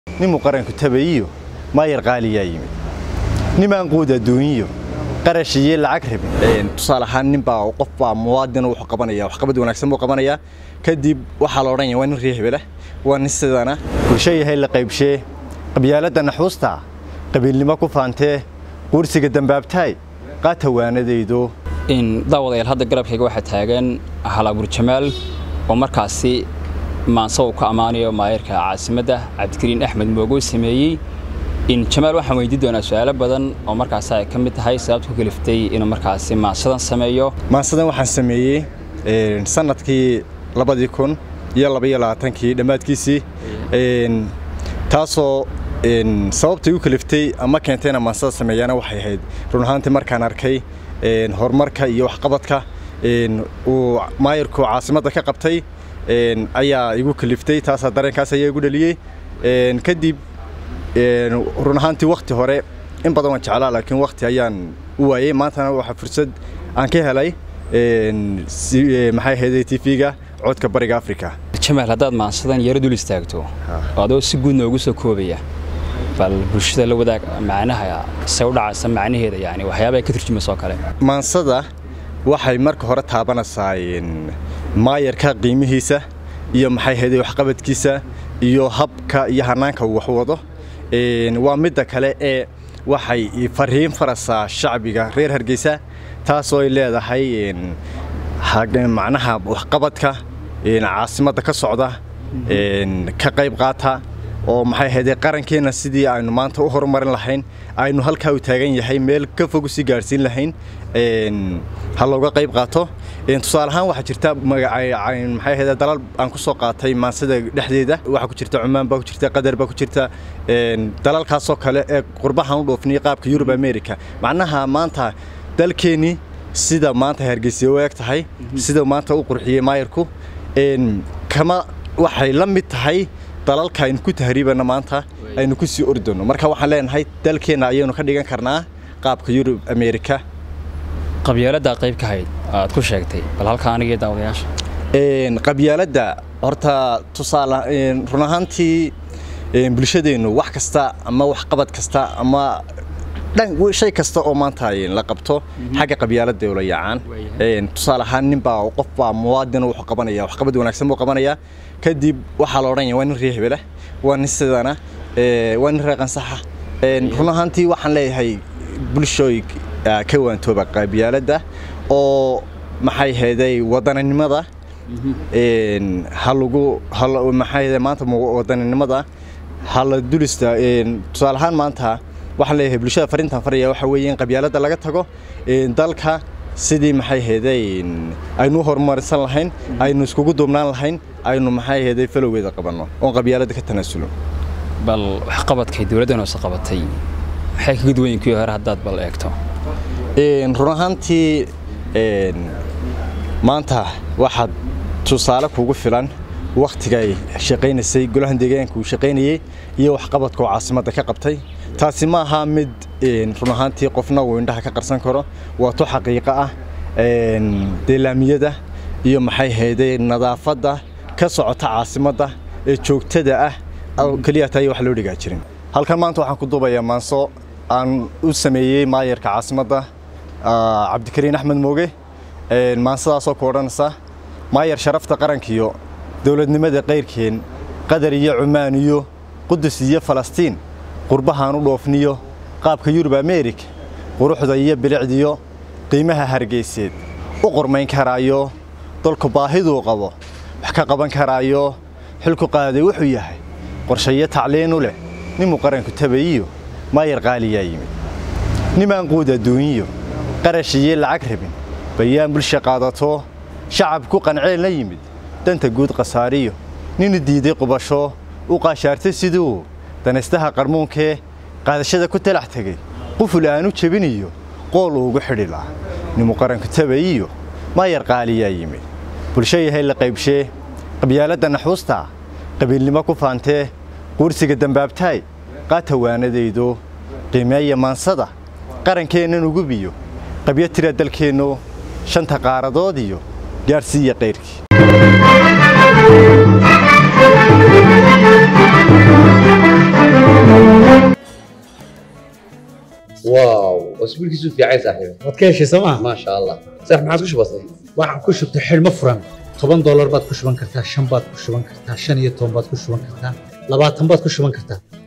Hal'abuur Timo Yilix oo maanso u tiriyay Duqa Hargeysa, C.kariin Axmed Mooge.
Sidaad u aragtaa ammaata masuuliyiinta dowladda?